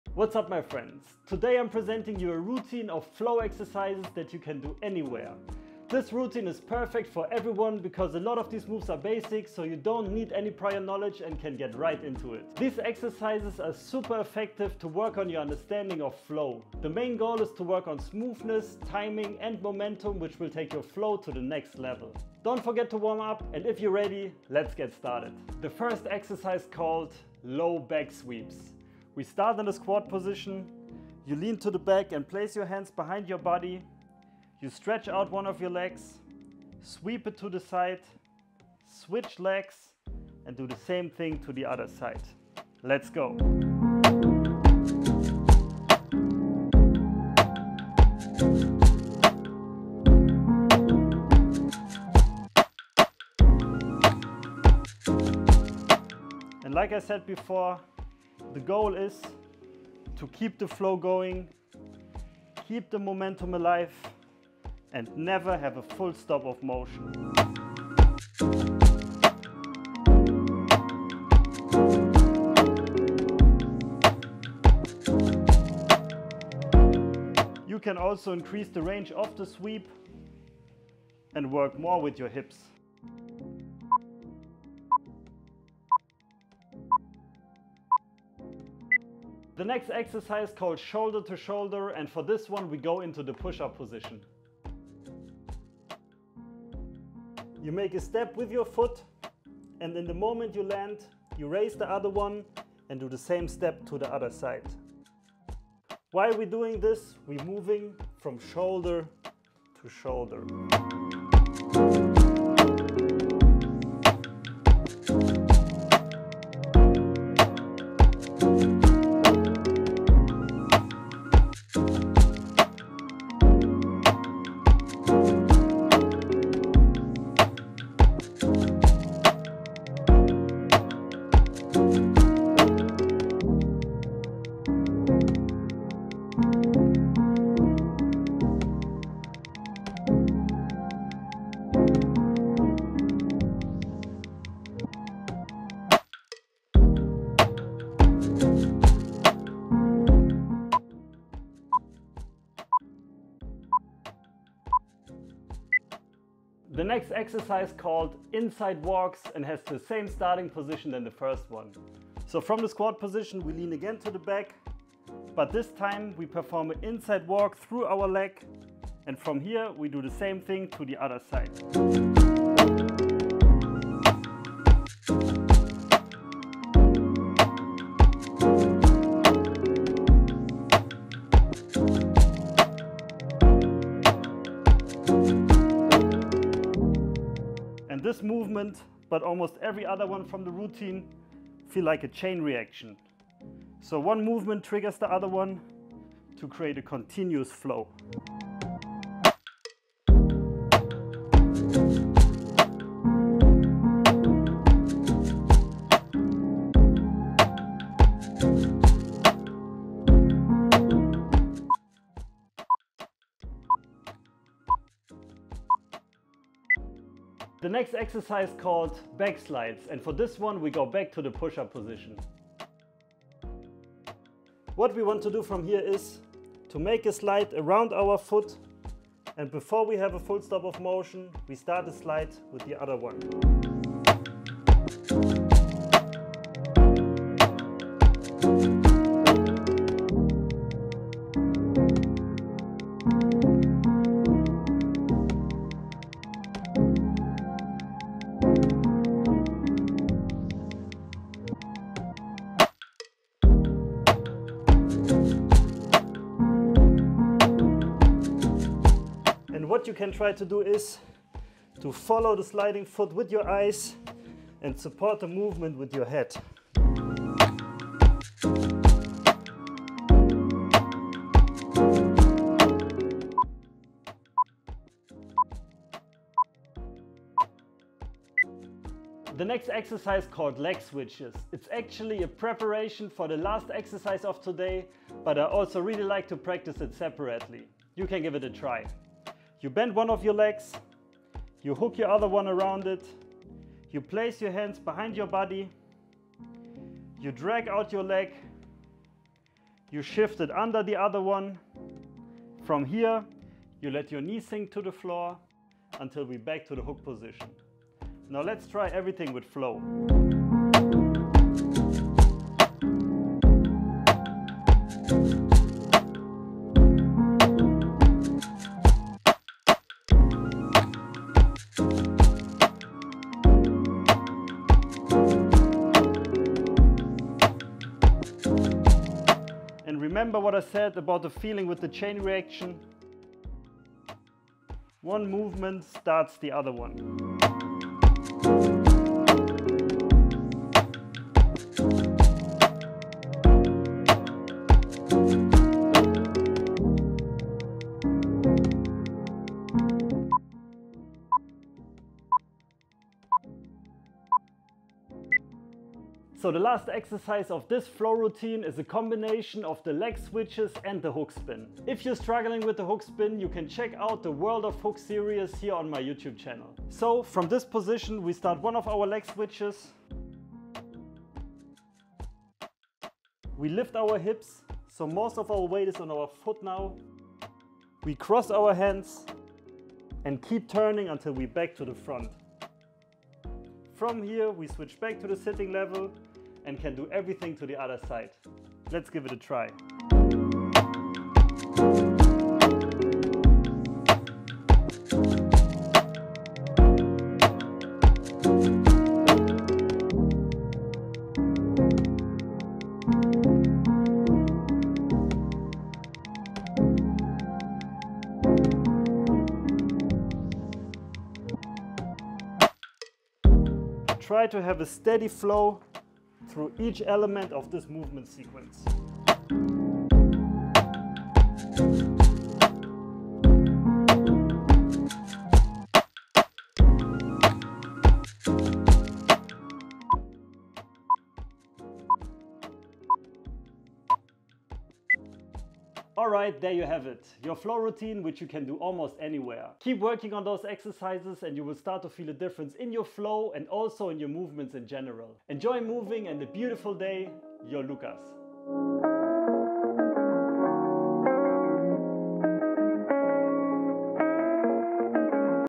DO THIS EVERYDAY TO FLOW ON THE GROUND🌊 5 MINUTES Flow routine🔥 Follow along 🙌🏼
Practice this routine everyday to build up the right feeling for flow in your movements. They will help you with your smoothness, timing and momentum, which are key factors to take your flow to the next level.

Thank you a lot for watching. I appreciate you a lot!

00:00 - 00:25 Introduction
00:25 - 01:30 Low back sweeps
01:30 - 02:37 Shoulder to shoulder
02:37 - 03:38 Inside walks
03:38 - 04:39 Back slides
04:39 - 05:56 Leg switches
05:56 - 07:16 Leg switches + Hookspin
07:16 - 07:36 Outro

Much love,
Lukas